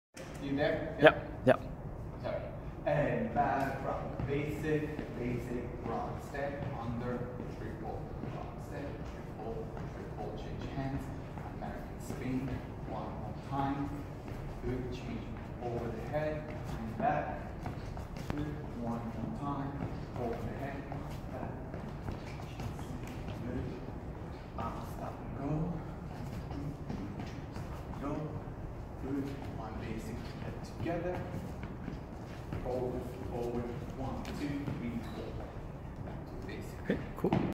Bronze Jive 2-6-18
Bronze Jive routine from Tuesday, 2/6/18.
Steps covered: rock step, triple step, underarm turn, American spin, change of hands behind the back, change of place, stop and go, walks